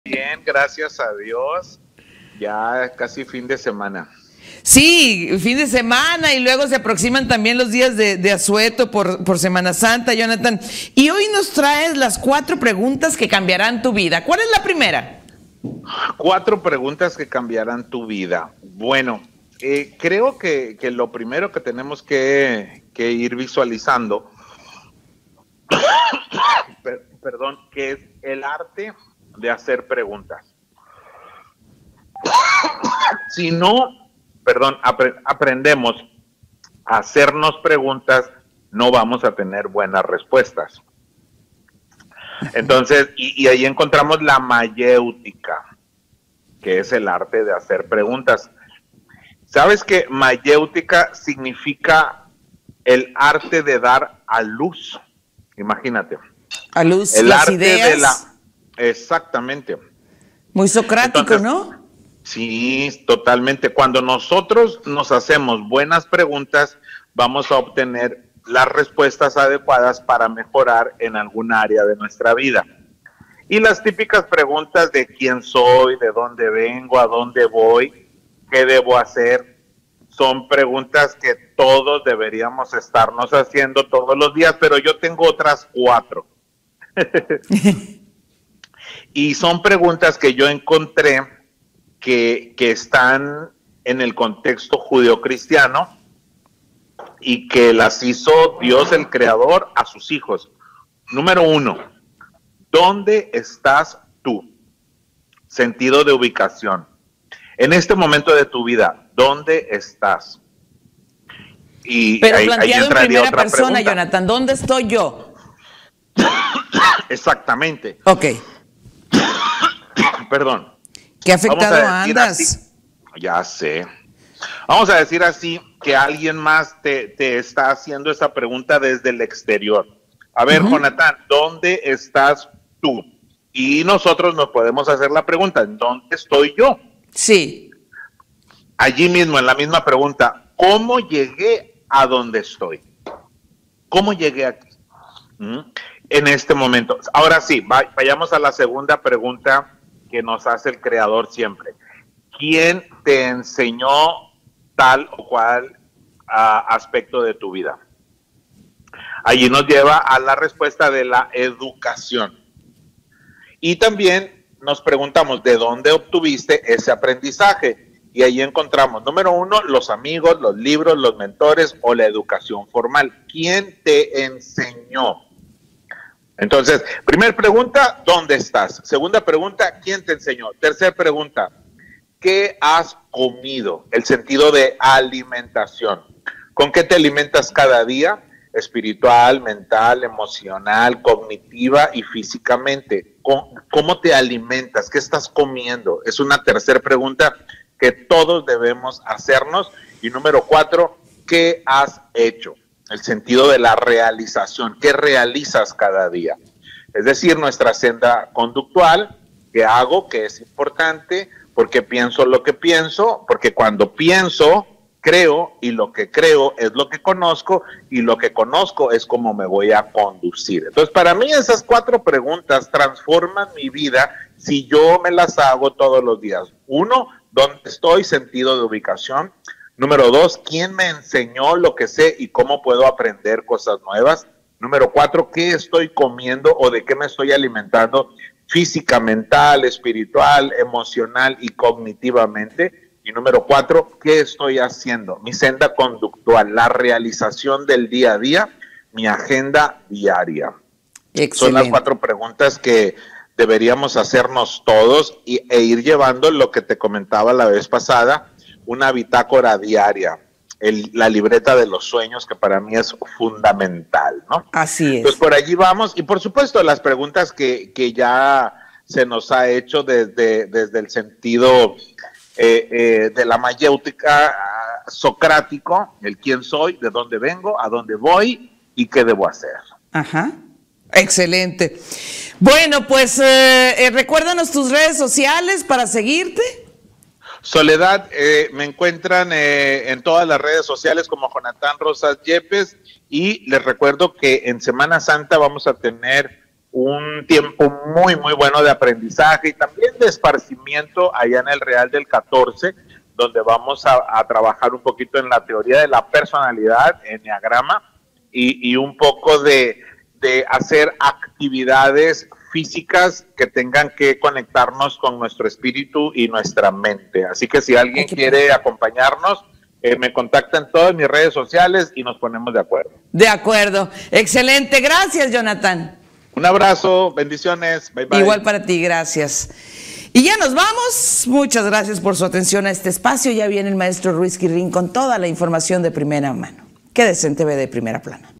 Cuatro preguntas que cambiarán nuestra vida.
🎤 En vivo con el Lic. Jonatan Rosas, nos habló sobre las cuatro preguntas que cambiarán nuestra vida.